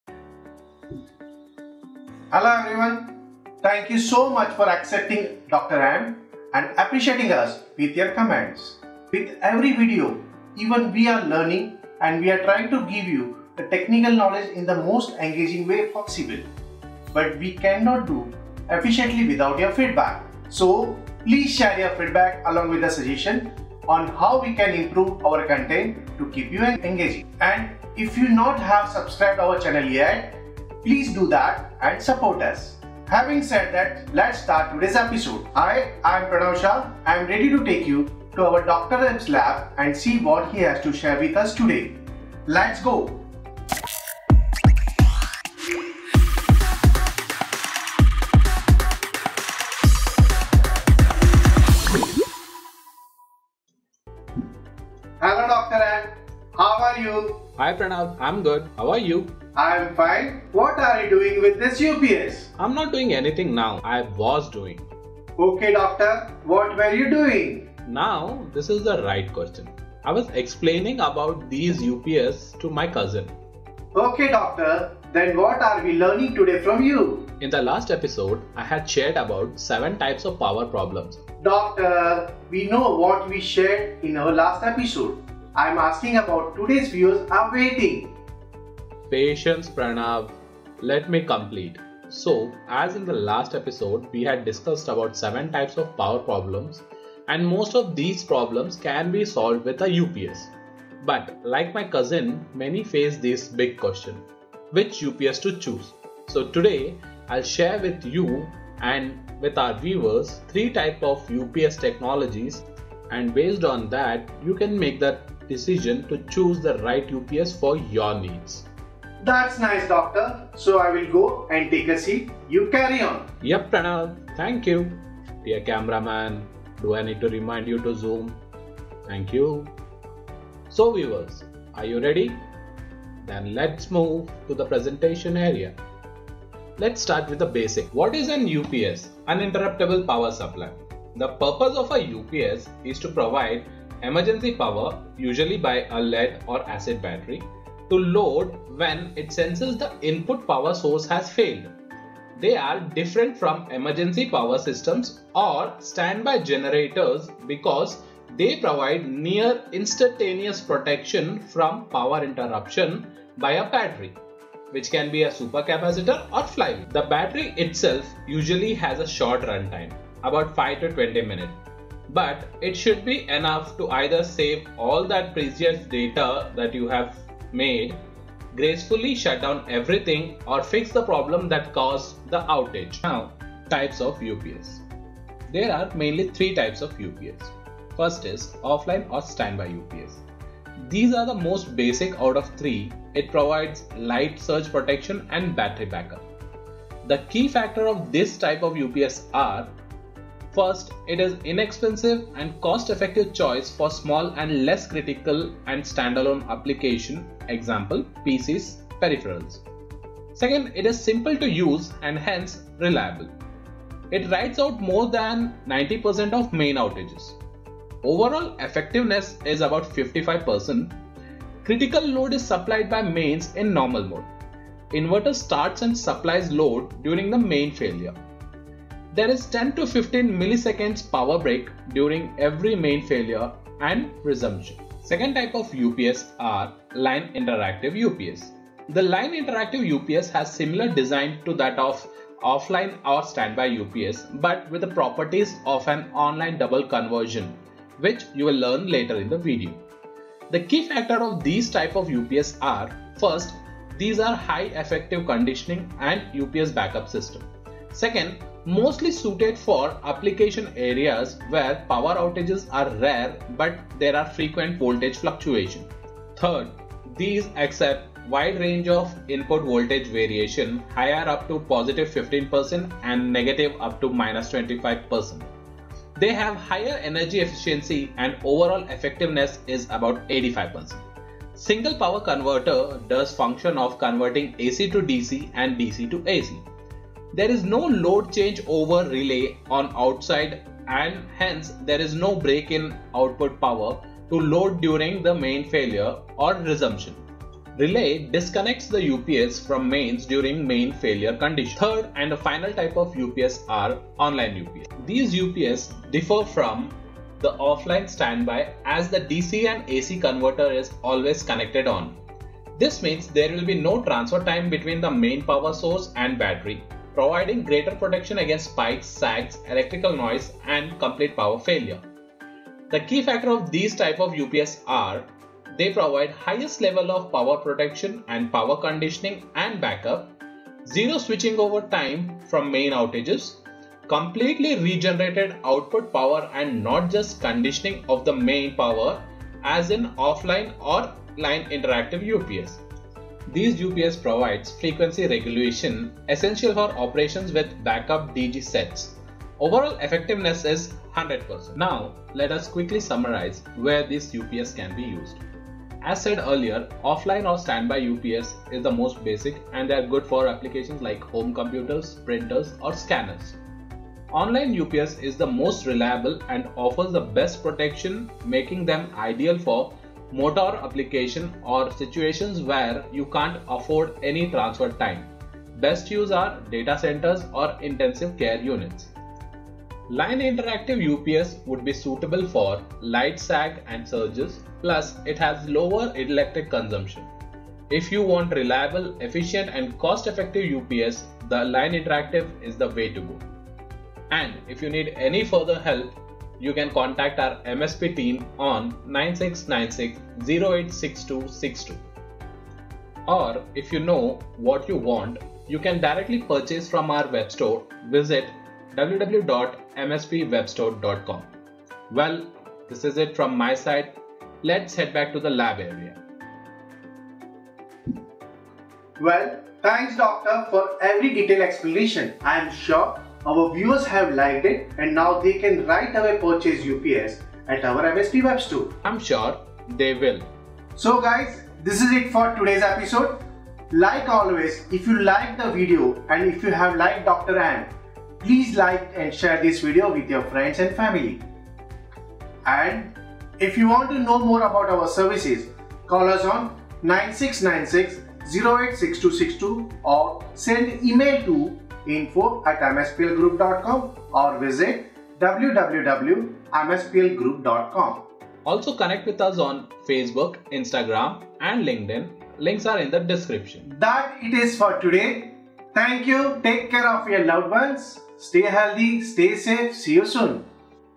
Dr Amp Ep2_Three Types of UPS System Technologies
What is a UPS -Uninterruptible Power Supply?

The purpose of a UPS is to provide emergency power (usually by a lead/acid battery) to a load when it senses that the input power source has failed. They are different from emergency power systems or standby generators because they provide near-instantaneous protection from power interruption by using a battery (which can be a supercapacitor or flywheel).

The battery itself usually has a short runtime (about 5-20 minutes), but it should be enough to either save all that precious data/progress that you have made, gracefully shut everything down, or fix the problem that caused the outage.

A UPS can be used to protect hardware like data centres, computers, and other electrical equipment where an unexpected surge/sag can cause serious problems like data loss, business disruption, and even injuries or fatalities.

Now Types of Uninterruptible Power Supplies

1. OFFLINE / STANDBY UPS.
The offline/standby UPS is the most basic out of the three. It provides light surge protection and battery back-up.

The key factor of this type of ups are

It is Inexpensive & Cost-effective choice for small and less critical & stand-alone application e.g. PC’s, Peripherals.
It is simple to use & hence reliable.
•  Rides out more than 90% of mains outages.
•  Overall effectiveness – 55%
•  The critical load is supplied by mains in normal mode
•  Inverter starts and supplies load during mains failure
•  There is “10 – 15 millisecond. Power Break” during every mains failure and resumption.

2. LINE INTERACTIVE UPS
The line-interactive UPS has a similar design to the offline/standby, but with properties of an online/double-conversion as well.

• Highly effective power conditioning and UPS backup system.
•  Mostly suited for application areas where power outages are rare but there are frequent voltage fluctuations.
•  Accepts a wide range of input voltage variation higher up to +15% to negative up to –25% Typ.)
•  They have Higher energy efficiency and Overall effectiveness is about – 85%
•  Single power converter does function of converting AC to DC and DC to AC.
•  There is no load changeover relay on the output side and hence there is no break in output power to the load during the mains failure or resumption.
•  Relay disconnects the UPS from mains during a mains failure condition.

3. ONLINE- UPS
  The online/double-conversion UPS differs from the offline/standby as the DC/AC inverter is always connected on. This means there will be no transfer time between the main power source and battery, providing greater protection against spikes, sags, electrical noise, and complete power failure.
•  Provides the highest level of power protection and power conditioning and backup.
•  Zero switchover time during mains outages.
•  Completely Regenerated output power and not just conditioning of the mains power as in Off-line or Line-Interactive UPS.
•  Provides frequency regulation (Essential for operation with backup D.G. sets)
•  Overall effectiveness – 100%

Item ID:                         HZGUFM4
Author Username:                 HimProduction
Licensee:                        Pranav Shah
Registered Project Name:         MSP TV
License Date:                    November 20th, 2020